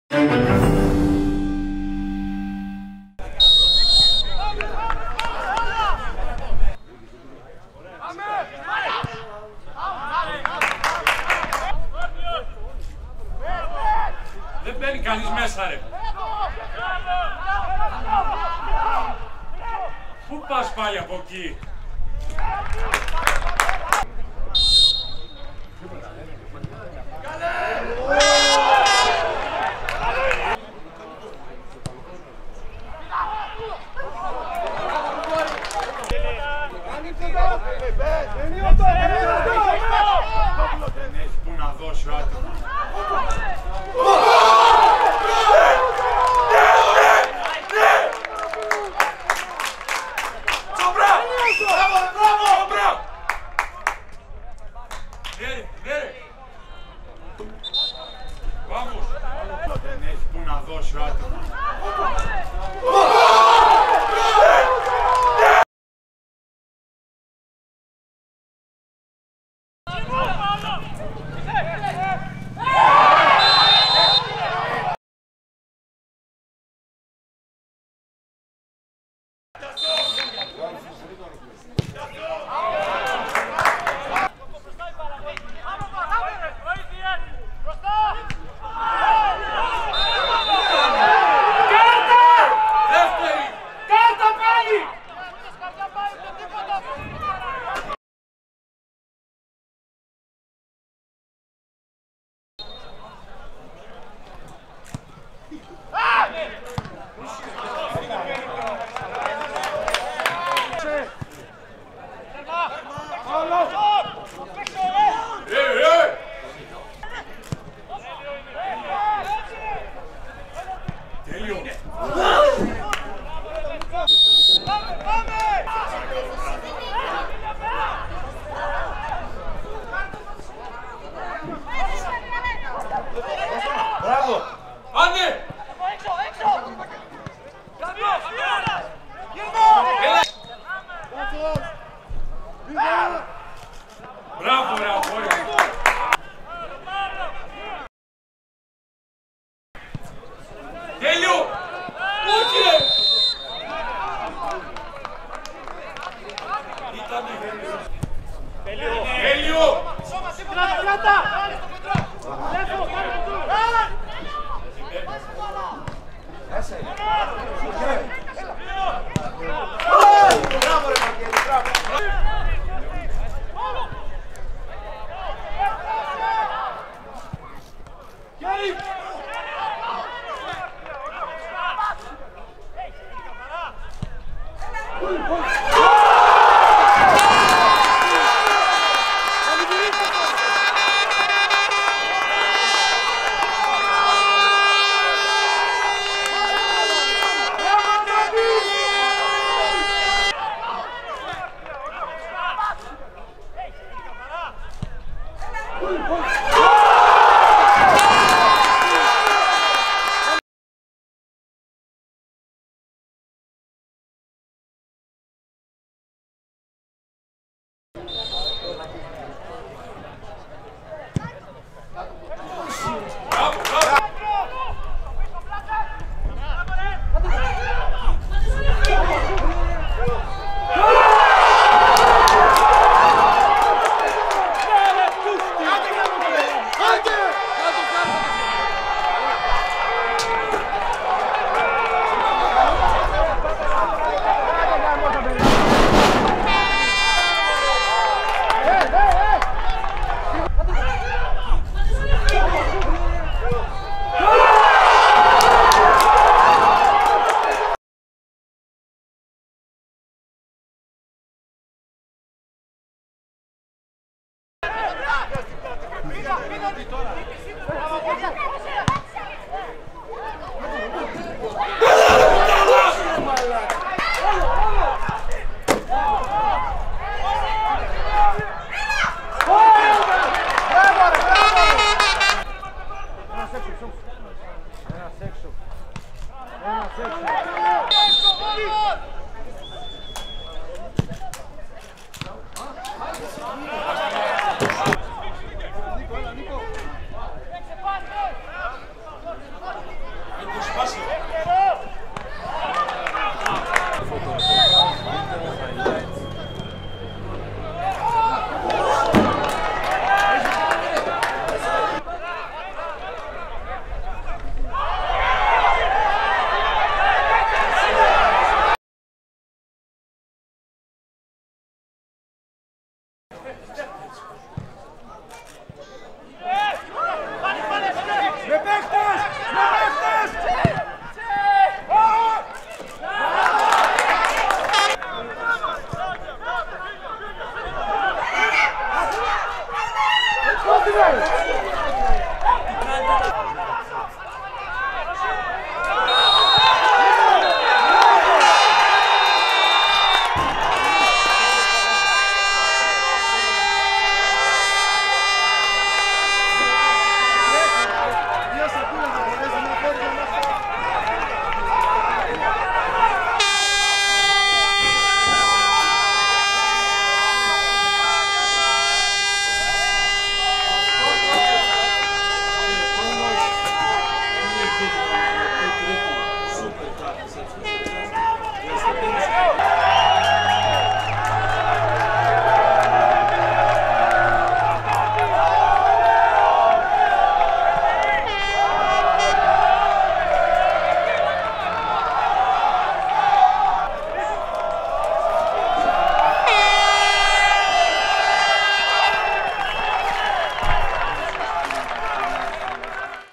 3o Super Cup ΕΠΣ Καστοριάς: Τα highlights του αγώνα Αστραπή - Καλλιθέα (2-1)
Οι καλύτερες στιγμές και οι αμφισβητούμενες φάσεις της αναμέτρησης στο γήπεδο της Πενταβρύσου, μεταξύ της Αστραπής Μεσοποταμίας και του Μέγα Αλέξανδρου Καλλιθέας (2-1), στα πλαίσια του 3ου Super Cup της ΕΠΣ Καστοριάς.